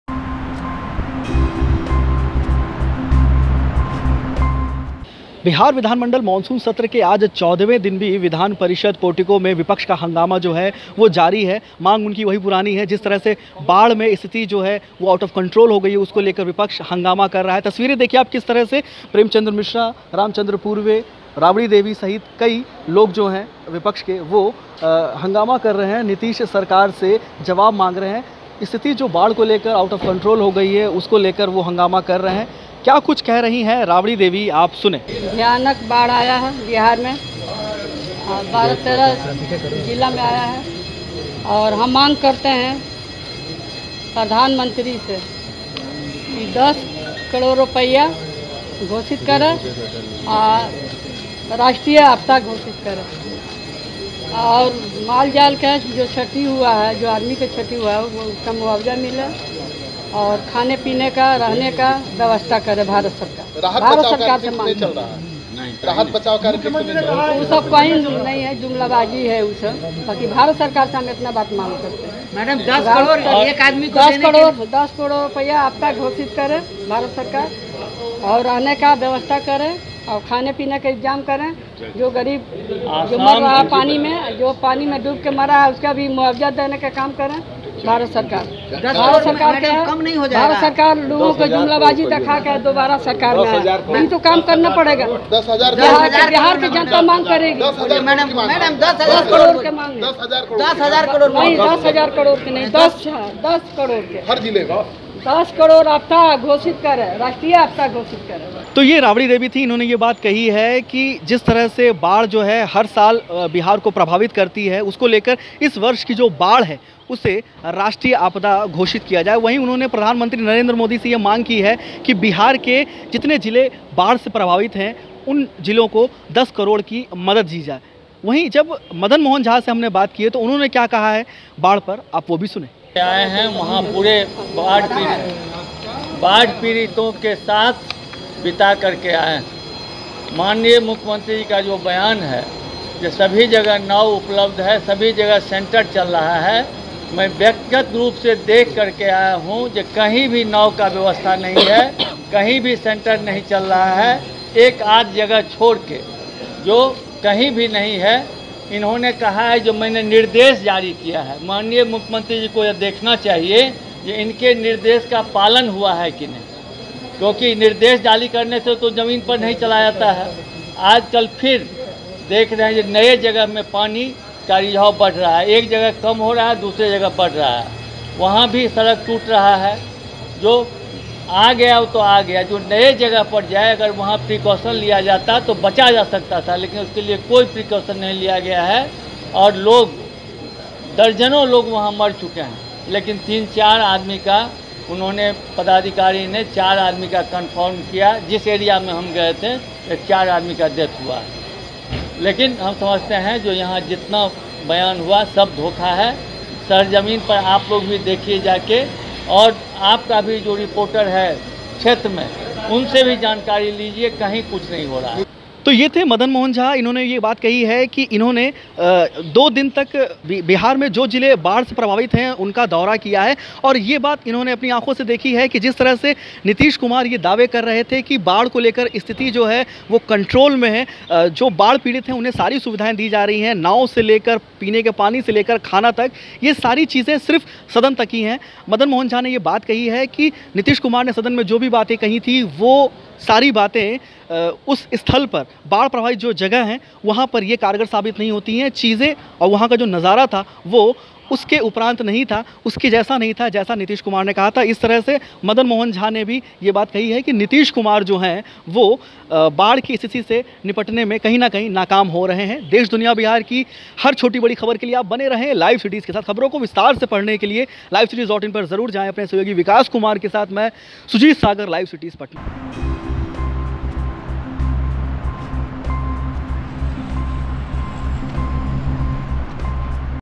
Bihar Flood पर Rabri Devi ने Modi Govt. से की बड़ी मांग, बोलीं - बाढ़ को करें राष्ट्रीय आपदा घोषित
Bihar Flood पर Rabri Devi ने Modi Govt. से की बड़ी मांग, बोलीं - बाढ़ को करें राष्ट्रीय आपदा घोषित | LiveCities